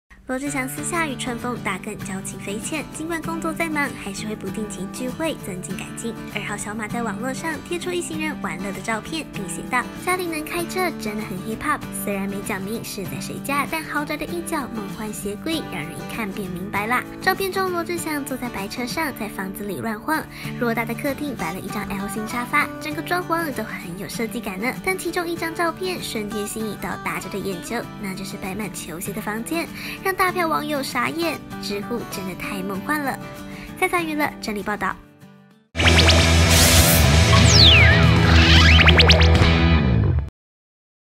【1年前】羅志祥在家開車超有膽！住宅公開“壕無人性”！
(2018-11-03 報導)

羅志祥在家開車超有膽！住宅公開“壕無人性”！